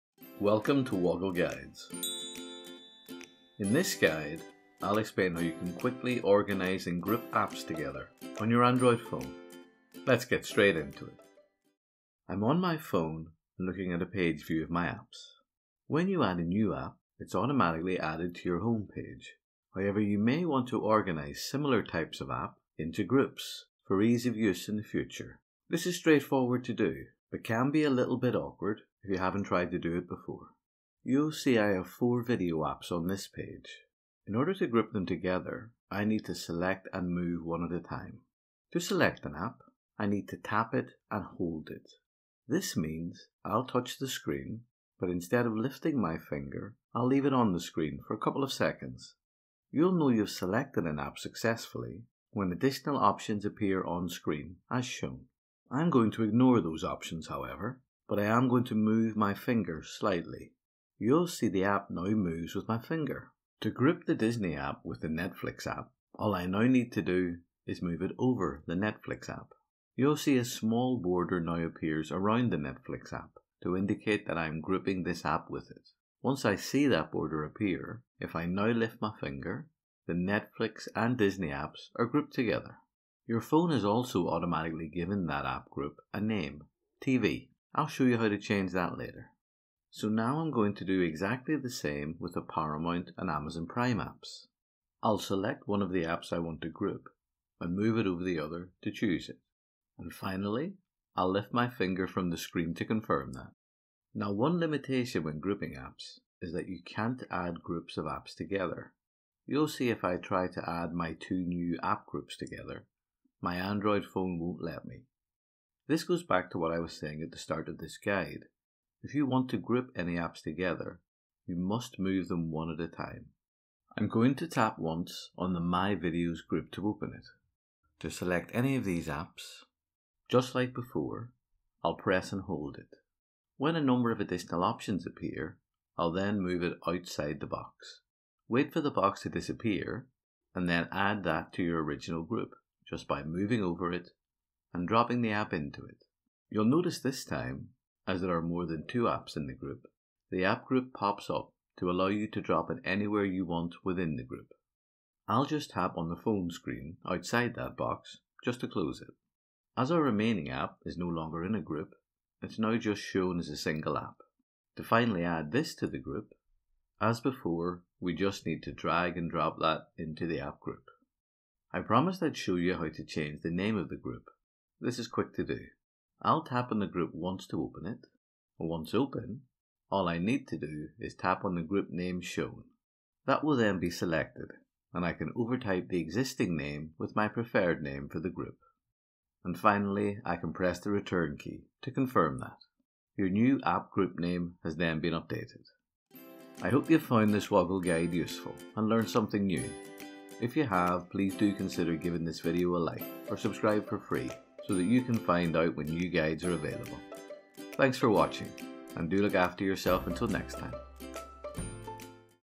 ORGANISE And Group Apps On A Google Android Phone
In this guide, I’ll explain how you can quickly organise and group apps together on your Android phone.

*🕔  Key Moments*
00:00 | Introduction
00:11 | How to organize and group apps on an Android phone
03:17 | Wrap up